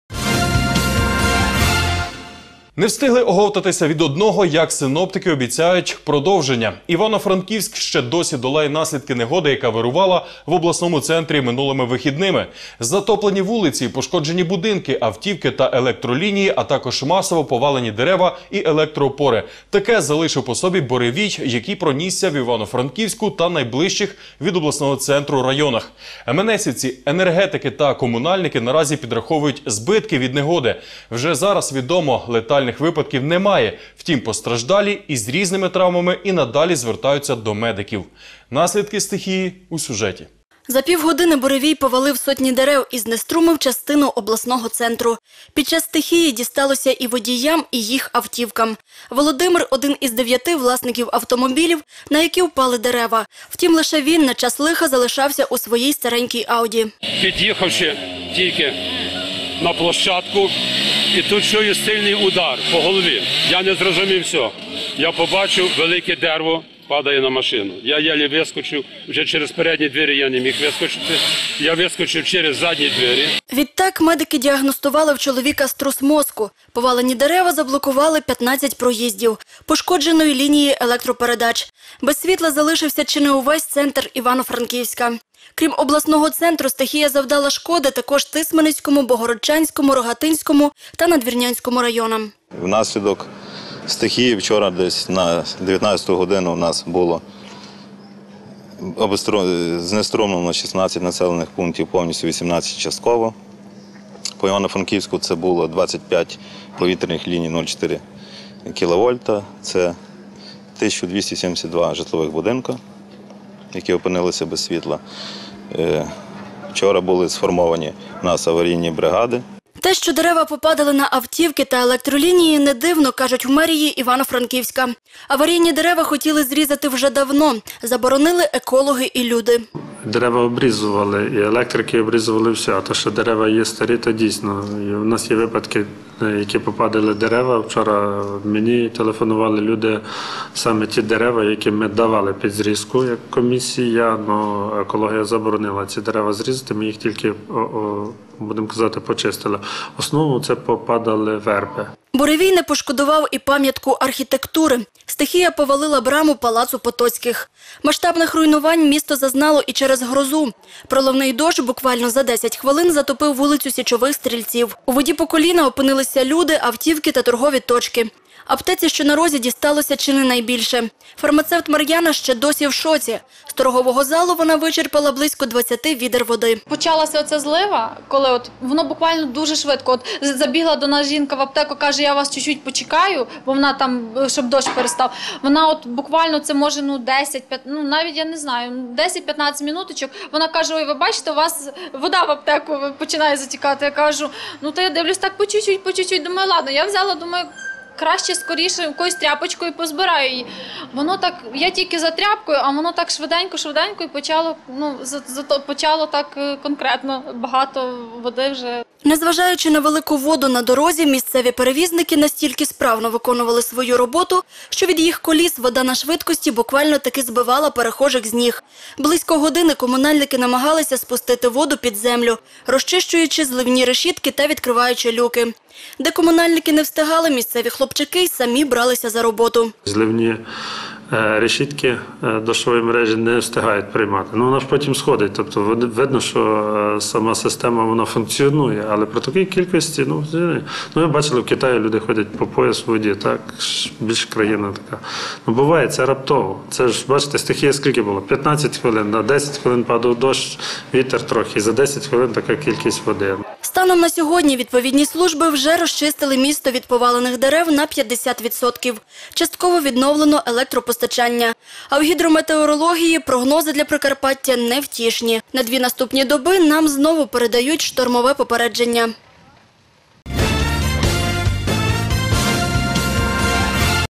Івано-Франківськ оговтується від стихії
Не встигли оговтатись від одного, як синоптики обіцяють продовження. Івано-Франківськ ще досі долає наслідки негоди, яка вирувала в обласному центрі минулими вихідними. Затоплені вулиці, пошкоджені будинки, автівки та електролінії, а також масово повалені дерева й електроопори - таке залишив по собі буревій, який пронісся у Івано-Франківську та найближчих від обласного центру районах. Еменесівці, енергетики та комунальники наразі підраховують збитки від негоди. Вже зараз відомо, летальних випадків немає, втім постраждалі із різними травмами й надалі звертаються до медиків. Наслідки стихії - у сюжеті.